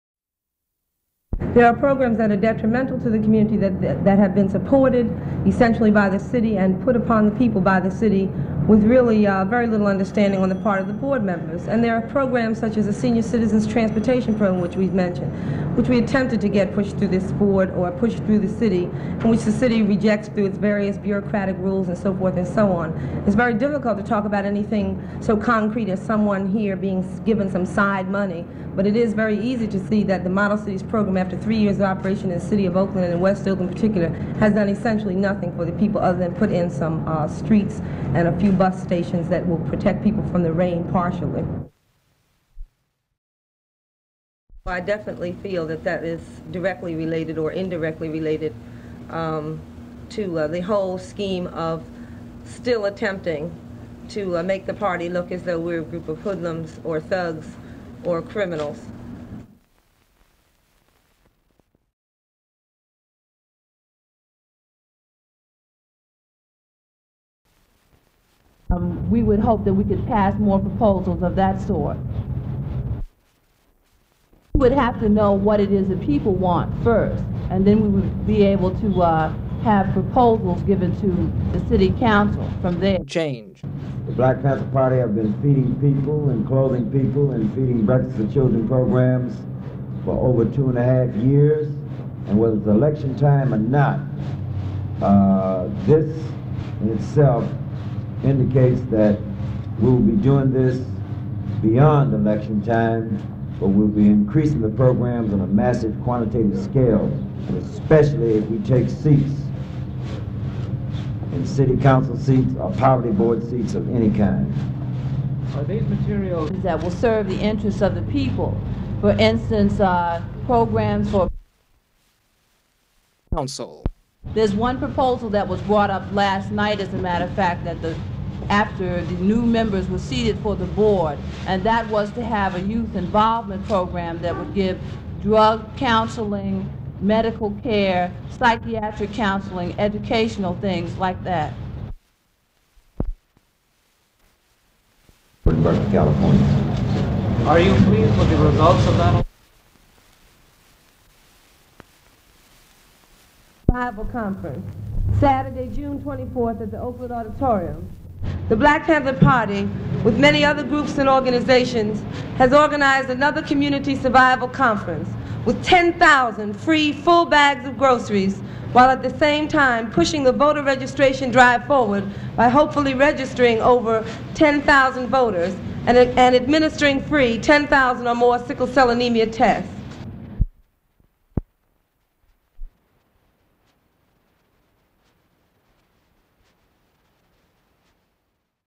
Elaine Brown Footage Compilation (1970s)
Press interviews with Black Panther Party Chairwoman Elaine Brown filmed in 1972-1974. Also featuring Ericka Huggins and Bobby Seale.